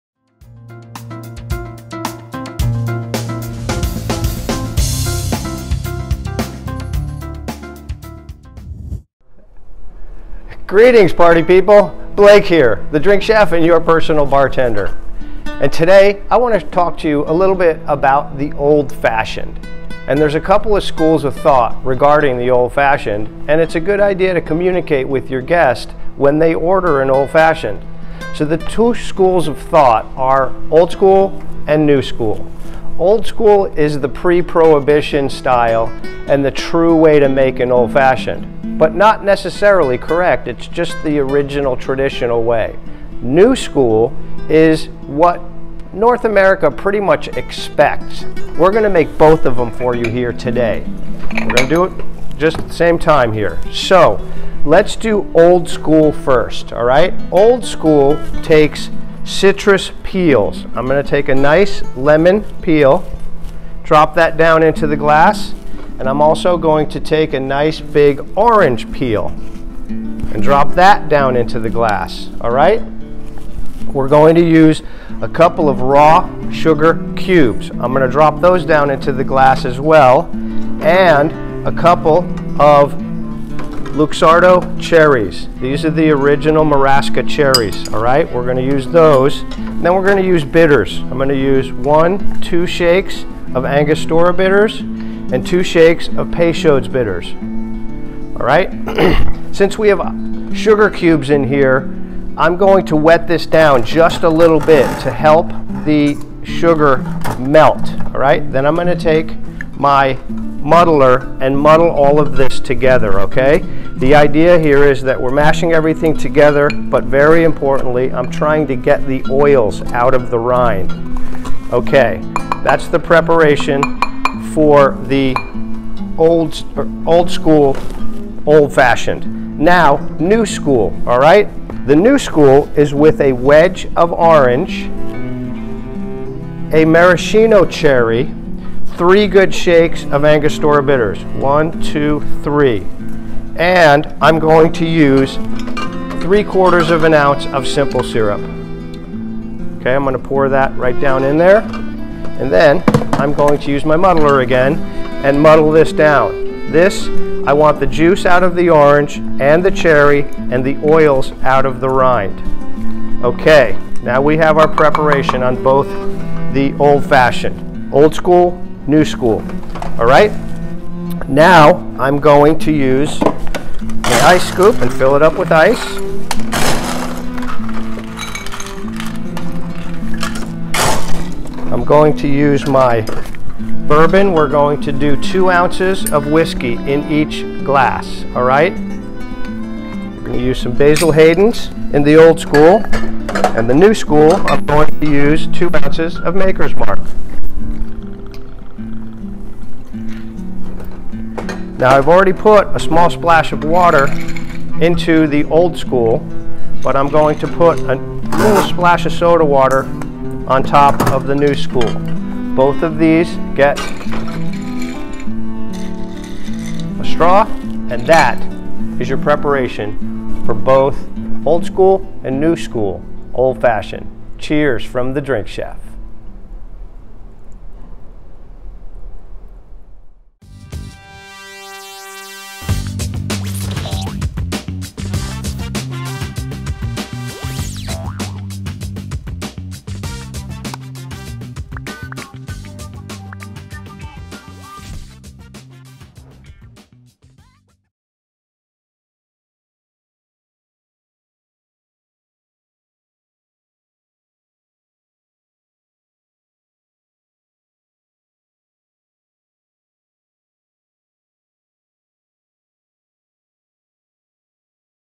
Old Fashioned 'Old School and New School'
This delicious concoction of whiskey, sugar, bitters, and citrus is quite literally the cocktail that started it all. It was that particular recipe that appeared alongside the first-ever printed use of the word 'cocktail' in 1806. And 80 years later, the name “Old Fashioned” started getting tossed around in bars to describe said cocktail recipe. Yes, even the old-timers of the 1880s considered it to be an old-school drink.

There are many ways to prepare this drink, and mine is not the end all. it's just 'this' is how I learned it, from my Pappy! Enjoy!